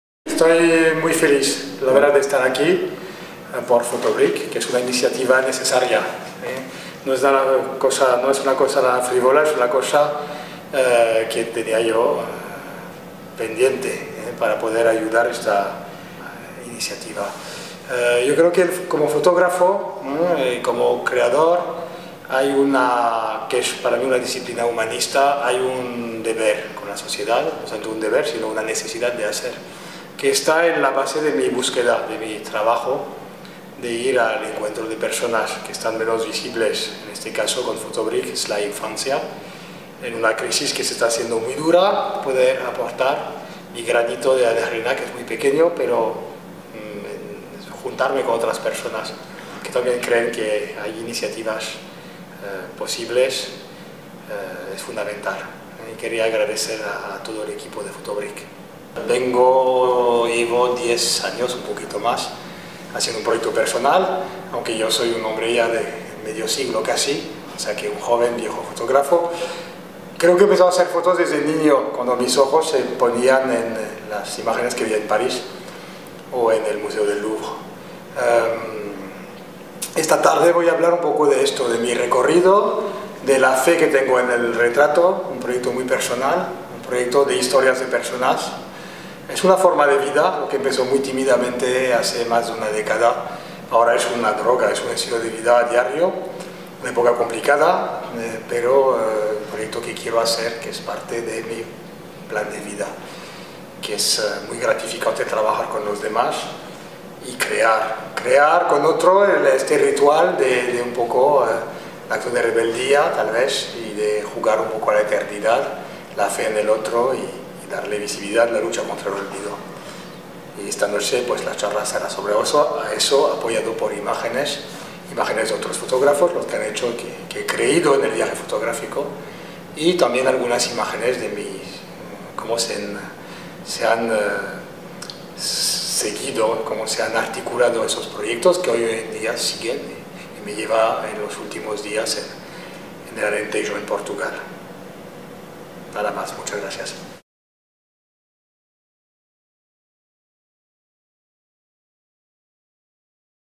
PHOTOBRIK 2-Pierre Gonnord
Pierre Gonnord participa en el programa "Photobrik", dando una conferencia en el Centro Atlántico de Arte Moderno (CAAM).

Las personas que asistan tienen que pagar como precio de entrada un brik (o más) de leche, que el CAAM donará al Banco de Alimentos de Las Palmas para su distribución entre personas con dificultades económicas.